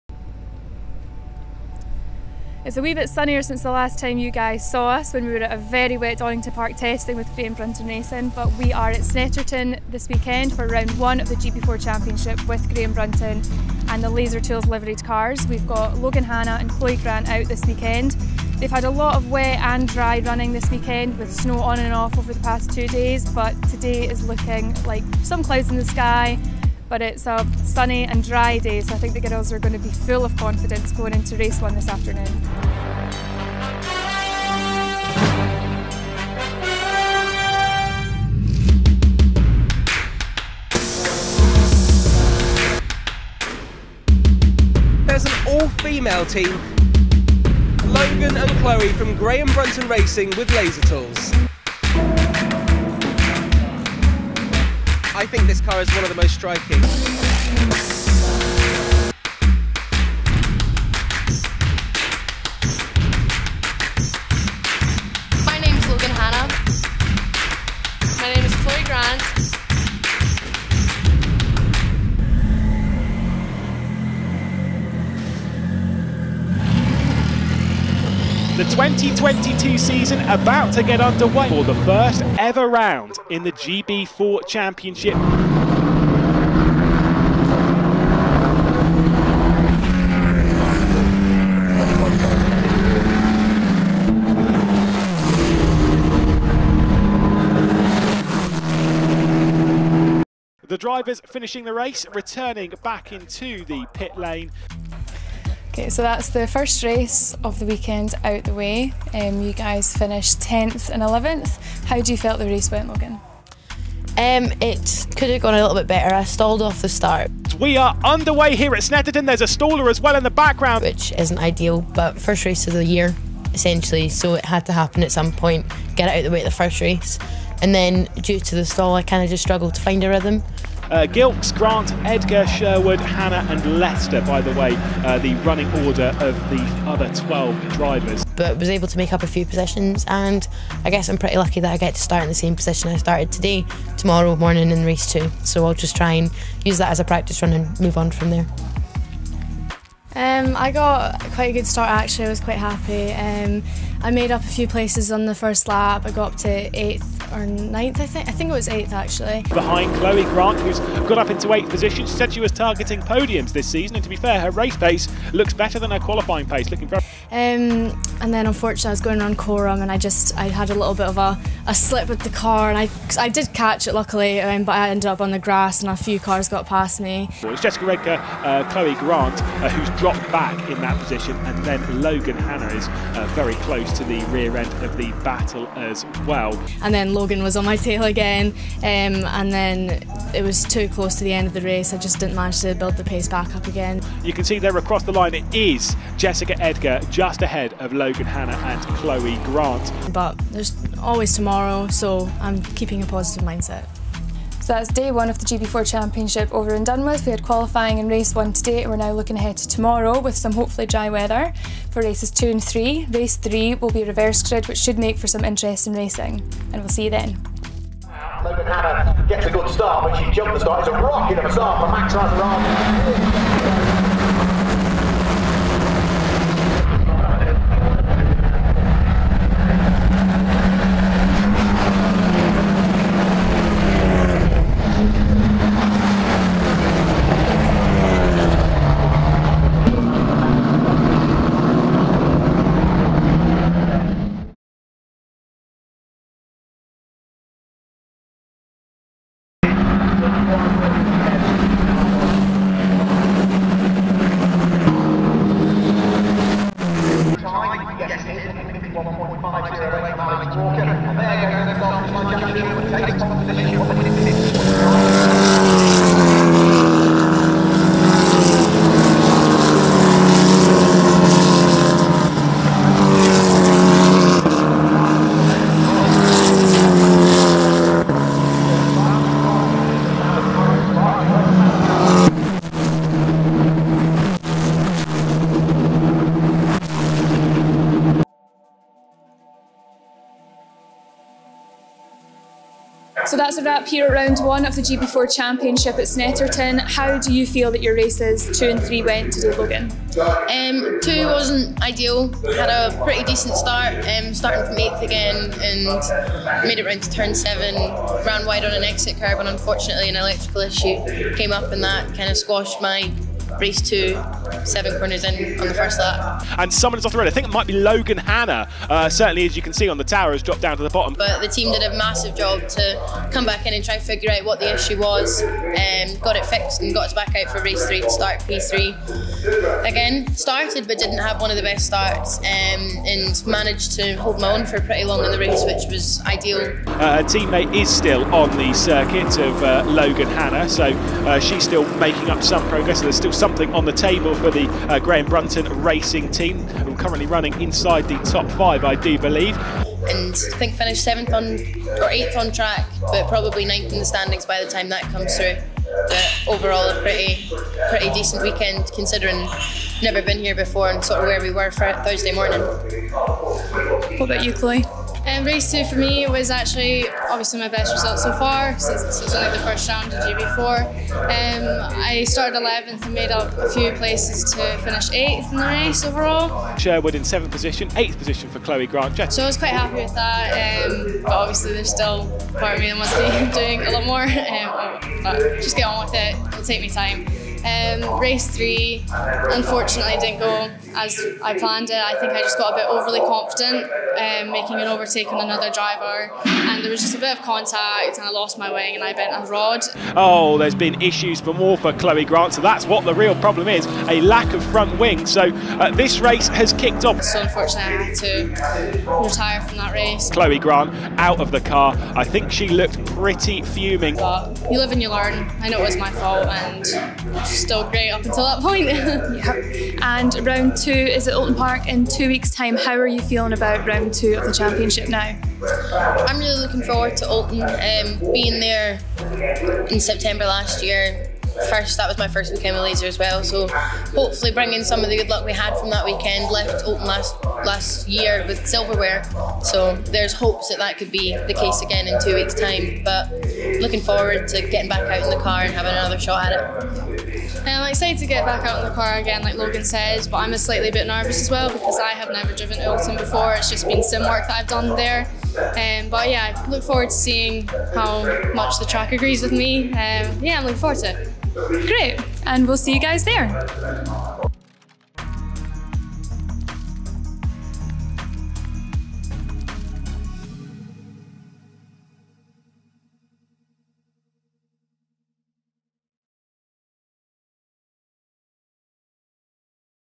Laser Tools Racing Report | 2022 GB4 Championship - Snetterton | 02-03 April 2022
The very first round of the GB4 Championship took place last weekend at Snetterton in Norfolk, and we were there with Laser Tools Racing drivers Logan Hannah and Chloe Grant. Here Lucy Grant interviews them the two across the weekend to find out how they got on, and how Chloe felt after her very first single seater race.

🎥 Video by The Laser Tools Film Unit.

🗓 2nd - 3rd April 2022.